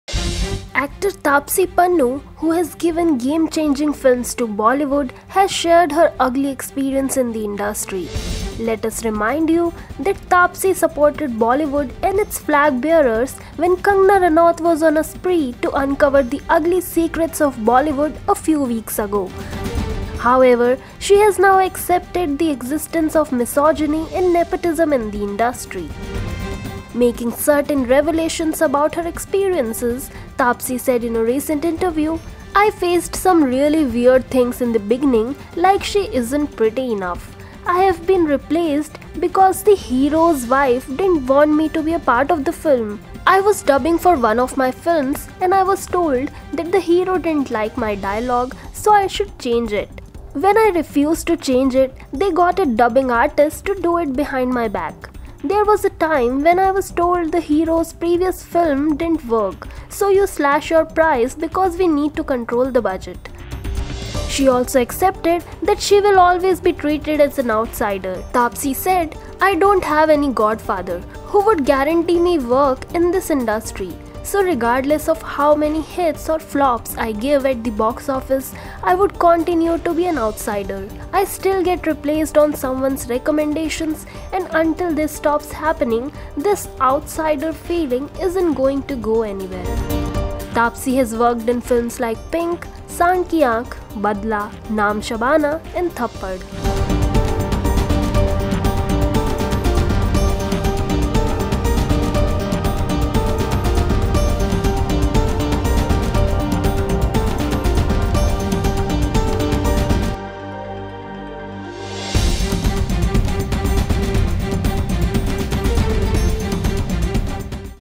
Taapsee Pannu Shares Her Ugly Experience In Bollywood
In a recent interview, Taapsee Pannu shared some ugly experiences she encountered in the Bollywood industry.